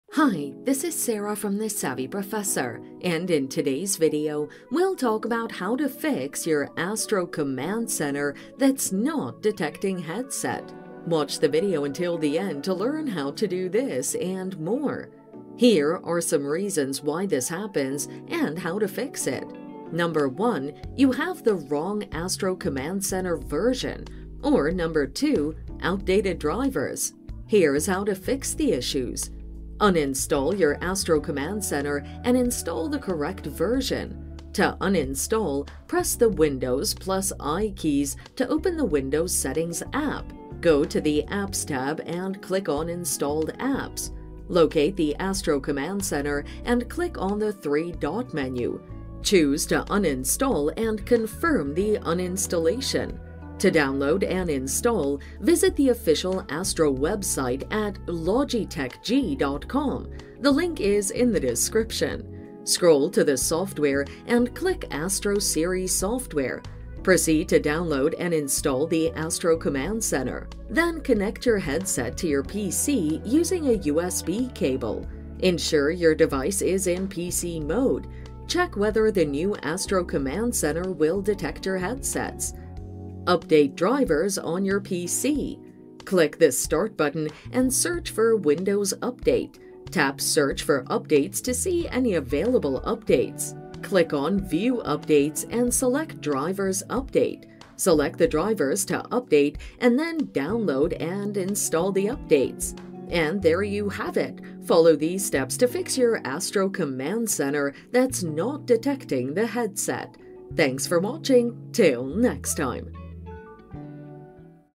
How To Fix It When Your Astro Command Center Is Not Detecting The Headset (A Comprehensive Guide!)
How To Fix It When Your Astro Command Center Is Not Detecting The Headset (A Comprehensive Guide!). In this video tutorial I will show you how to fix Astro command center is not detecting the headset.

Here are some reasons why this happens and how to fix it:
1. You have the wrong Astro Command Center version.
2. Outdated drivers.

Here is how to fix these issues:
Uninstall your Astro Command Center and install the correct version.
To uninstall;
• Press the Windows + I keys to open the Windows settings app.
• Go to the Apps tab and click on Installed Apps.
• Locate the Astro Command Center and click on the three-dot menu.
• Choose to uninstall and confirm the uninstallation.

• Scroll to the software and click Astro Series software.
• Proceed to download and install the Astro Command Center.
• Then, connect your headset to your PC using a USB cable.
• Ensure your device is in PC mode.
• Check whether the new Astro Command Centre will detect your headsets.

Update drivers on your PC.
• Click on the Start button and search for Windows update.
• Tap Search for updates to see any available updates.
• Click on View Updates and select Drivers Update.
• Select the drivers to update.
• Then, download and install the updates.